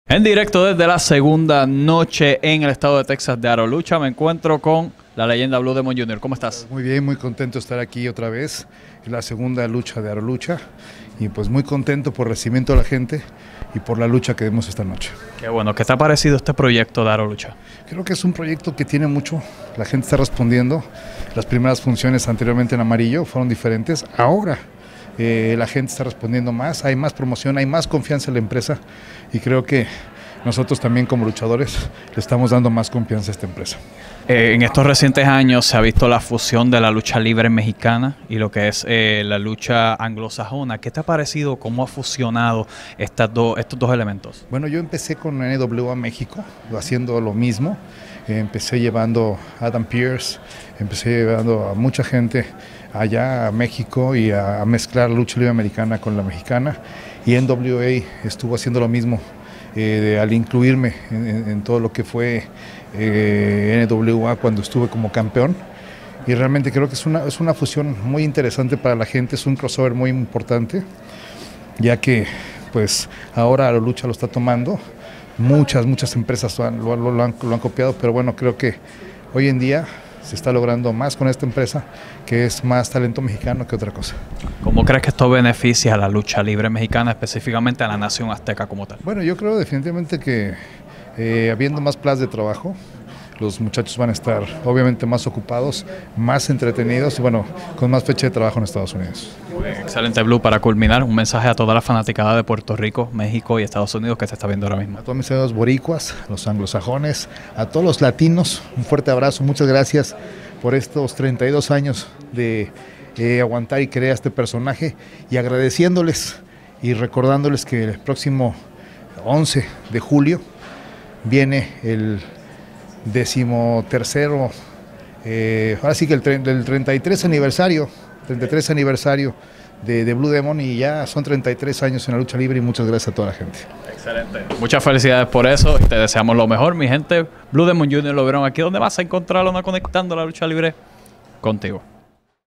Blue Demon Jr: "Fusionar estilos de lucha abre más plazas de trabajo en Estados Unidos"
El hijo de la leyenda azteca comenta sobre la fusión de la lucha libre mexicana y anglosajona dentro del proyecto Aro Lucha, cómo beneficia esto a los luchadores mexicanos, la celebración de su décimo tercer aniversario en la lucha libre, entre otros temas de interés.